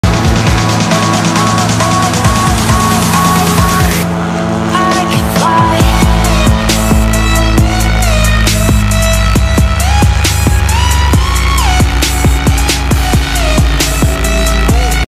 VALDEZ | Alaska STOL Competition | HELIO H-295 | Short TAKE OFF
1972 HELIO H-295 competes at the Valdez STOL Competition in Valdez Alaska.